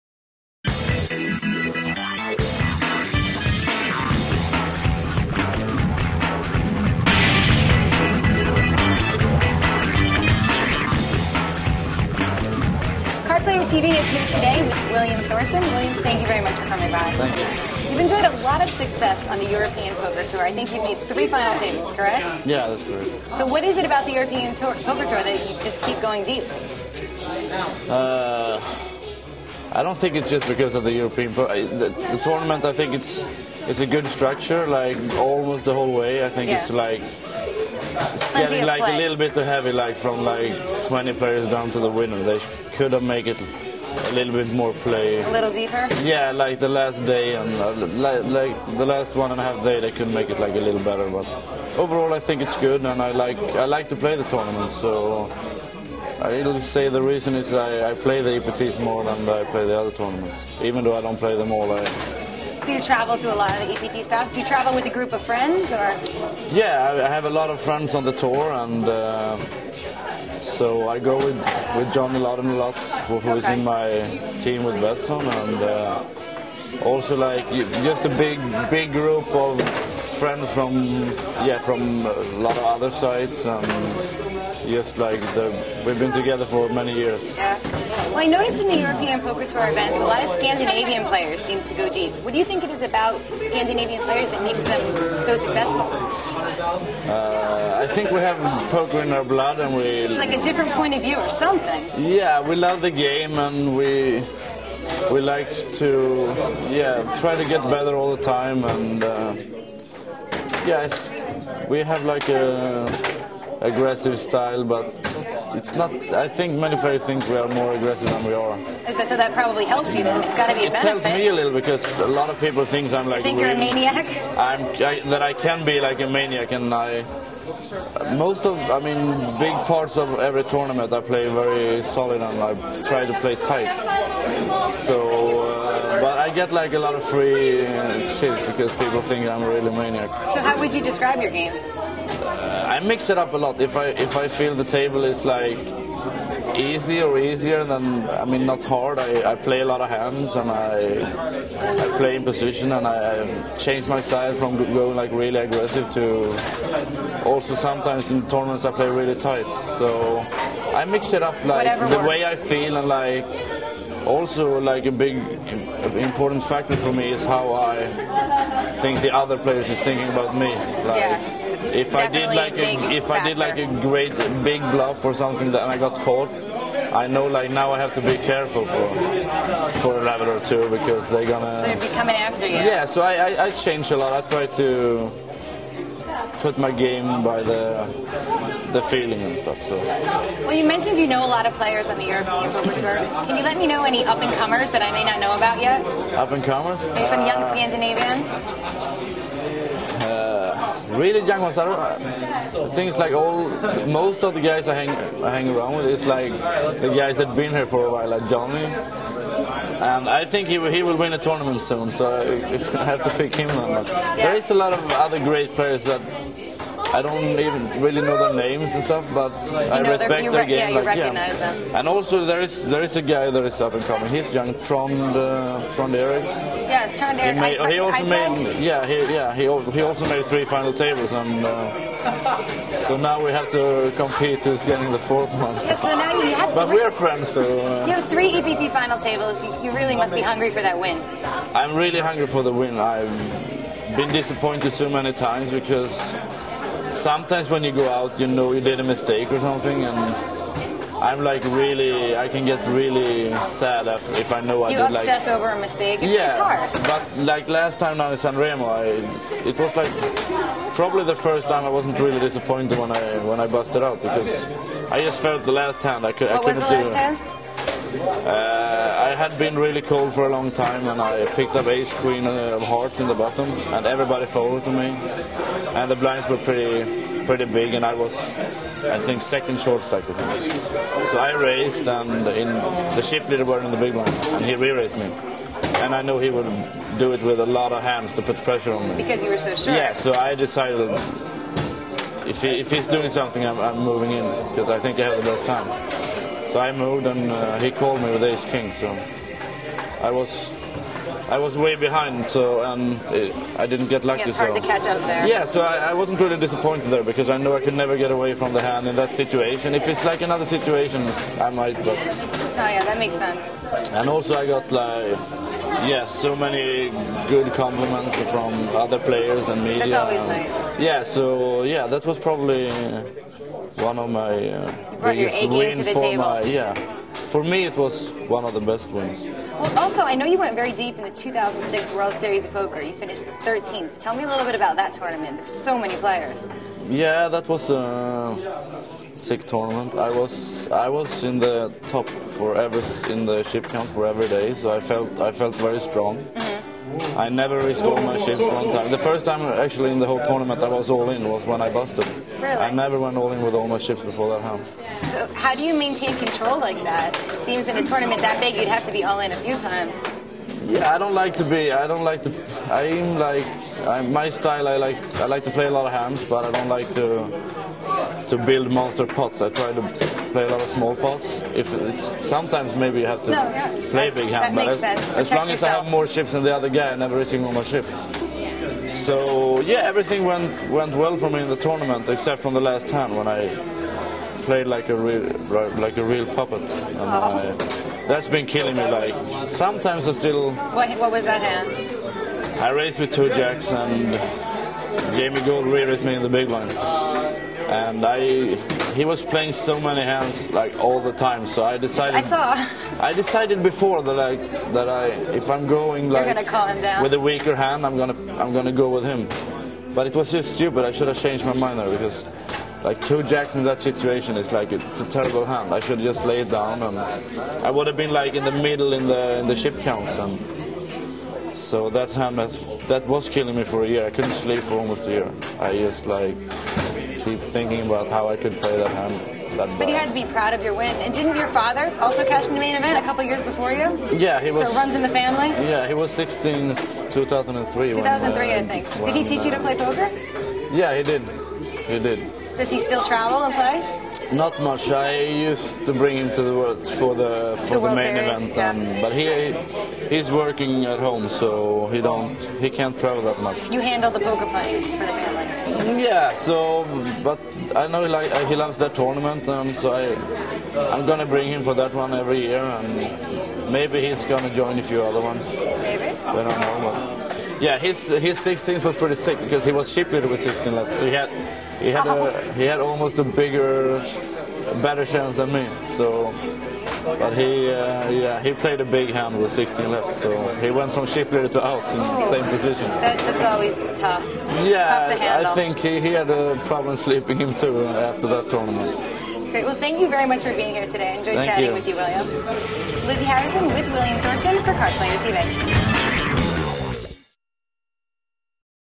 PokerStars.com EPT Grand Final: William Thorsson
Card Player TV talks to William Thorsson at the 2008 PokerStars.com EPT Grand Final.